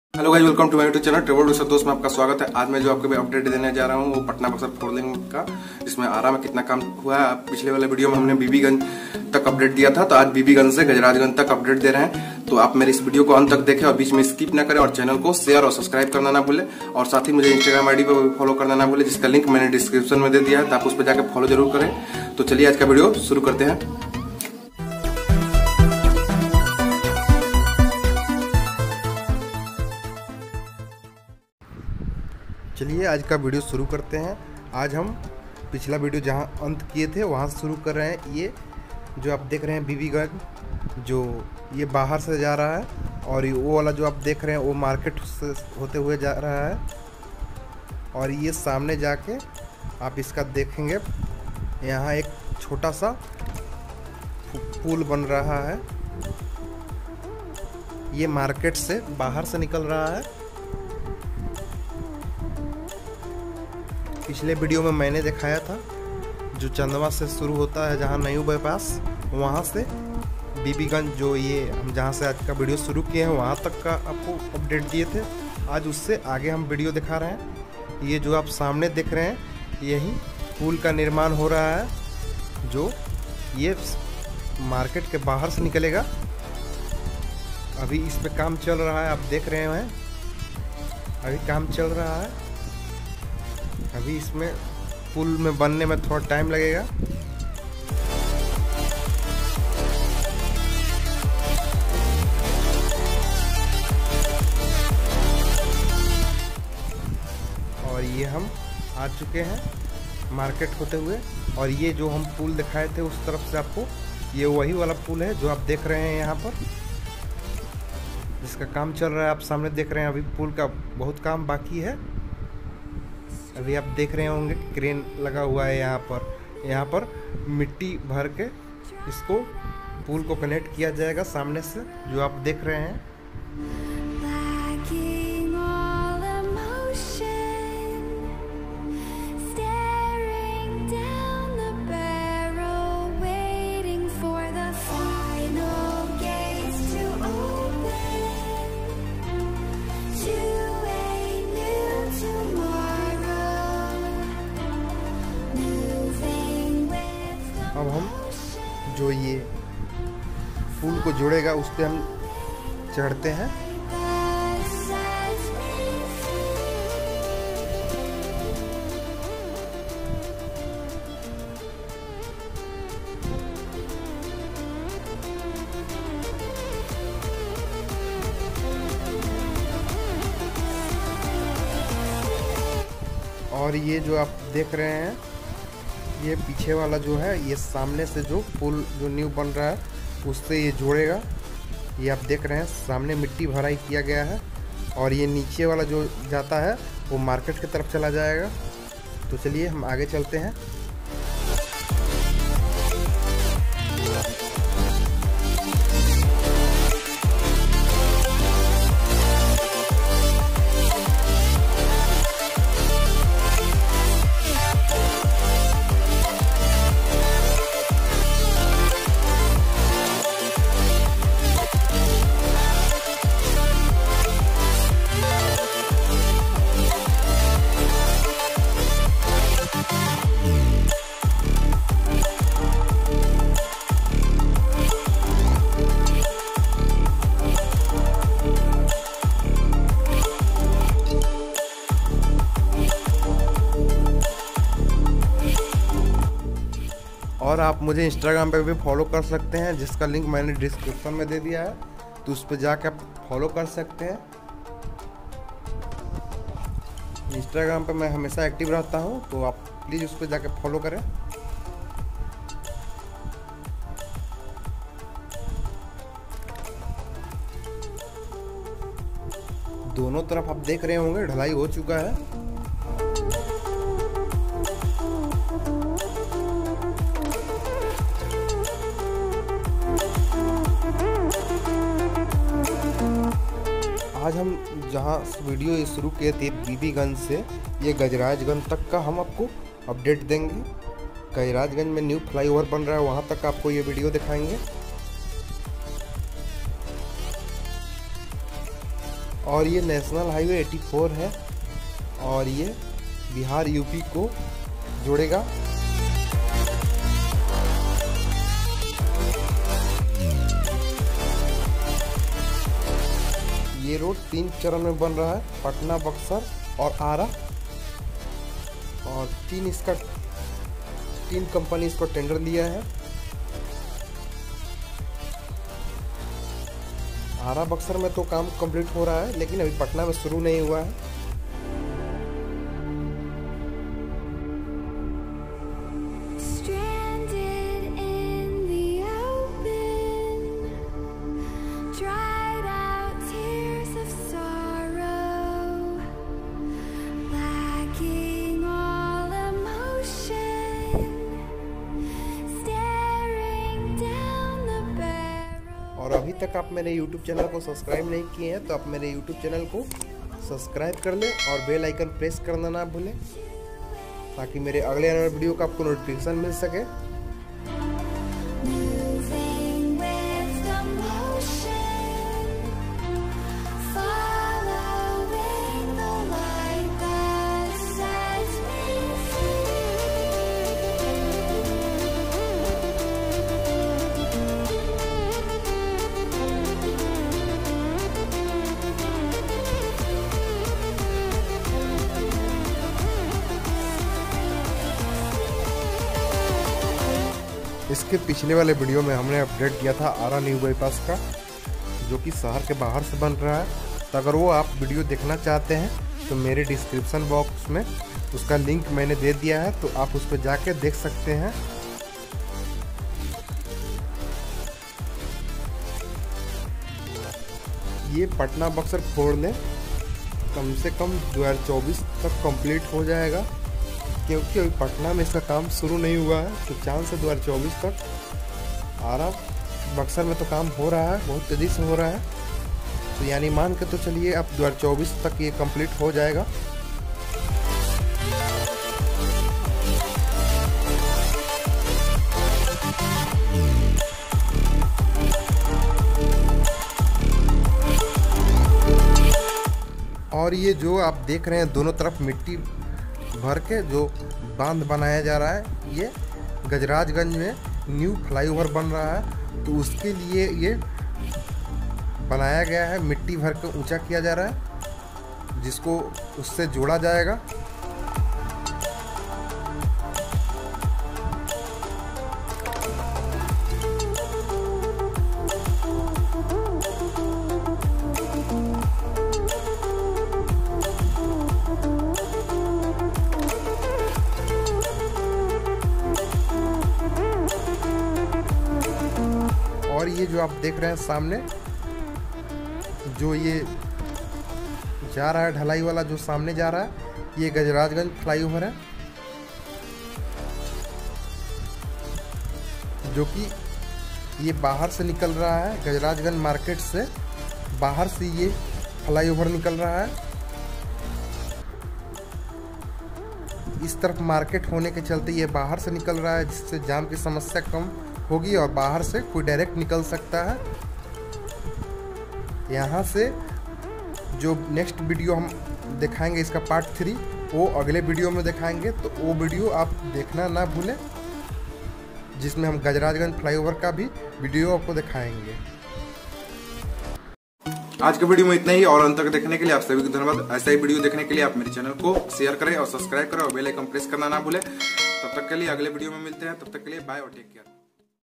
Patna -Buxar four lane project details | Arrah- buxar NH -84 Update | Bibiganj to Gajrajganj update
Patna -Buxar four lane project details |  Arrah- buxar NH -84 Update | Bibiganj to Gajrajganj update( Part 2)